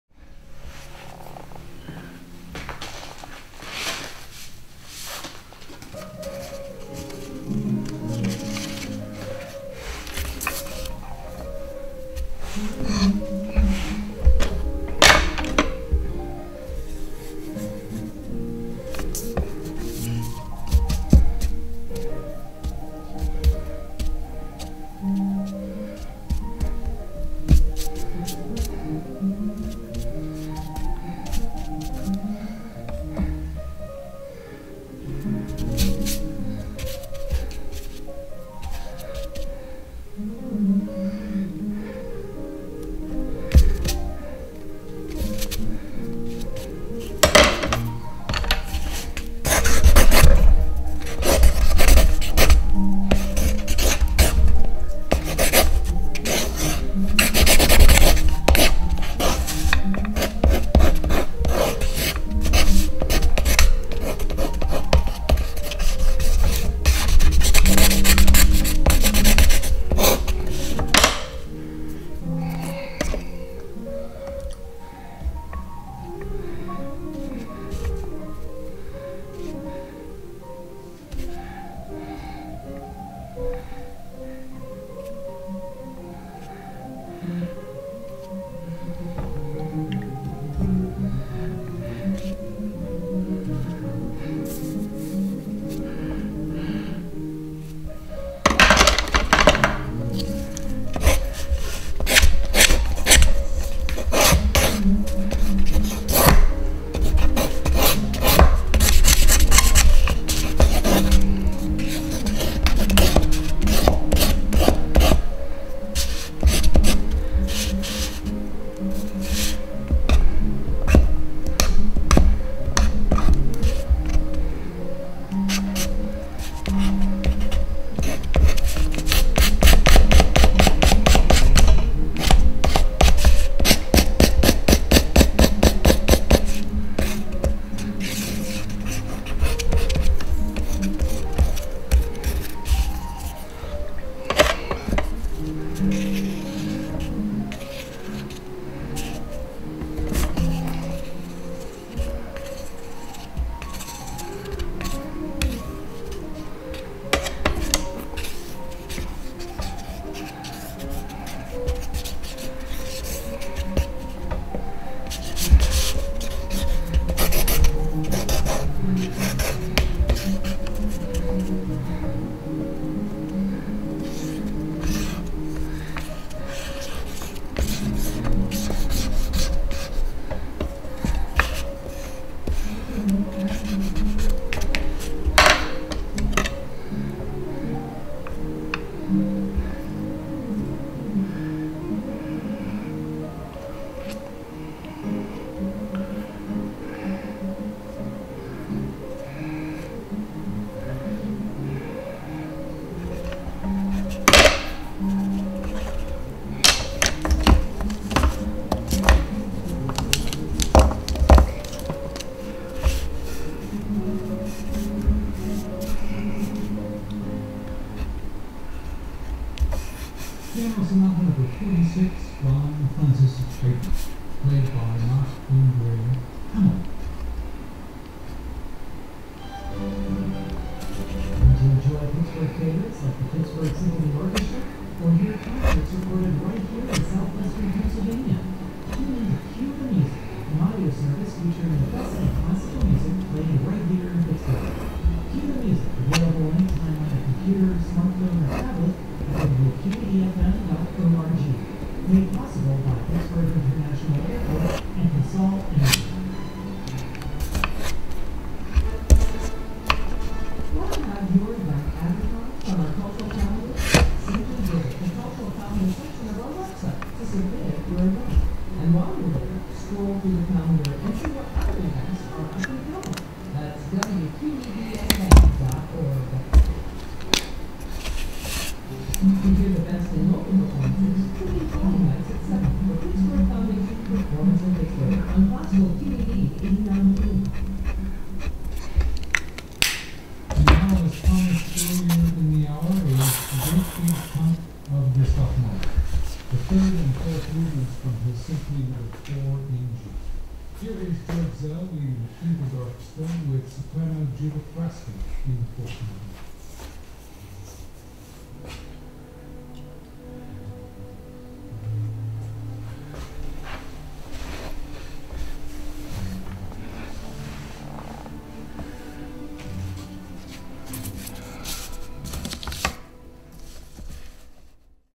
A live painting performance "Crawling into a Incense Burner" by Victor Beltran
"Crawling into a Incense Burner is five minutes watercolor painting by Victor Beltran at his Art Studio in Pittsburgh.
He said: "It is a five minutes journey to the center of my being to know what I want."